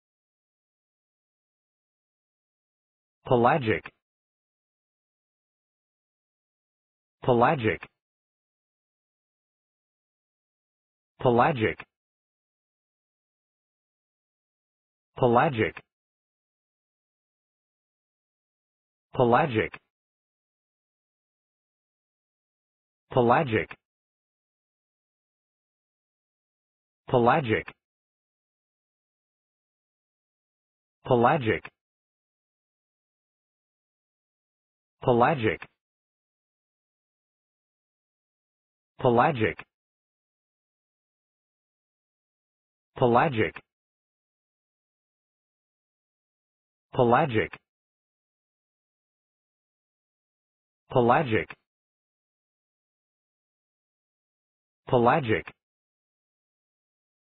Pronunciation of Pelagic | Definition of Pelagic
Pelagic pronunciation | How to pronounce Pelagic in English?
/pə`lædʒɪk/
Meaning of Pelagic | What is Pelagic?
(adjective) relating to or occurring or living in or frequenting the open ocean
What is Pelagic | Pelagic meaning and definition?